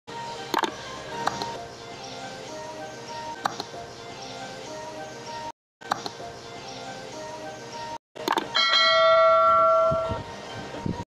How to Promote YouTube Channel ?
If you are in need of promoting your YouTube channel especially in the number of Subscribers. You are in a right Channel and video now.

Note: This is for LEGIT SUBSCRIBERS only, if you are not then please skip this video.

Thankyou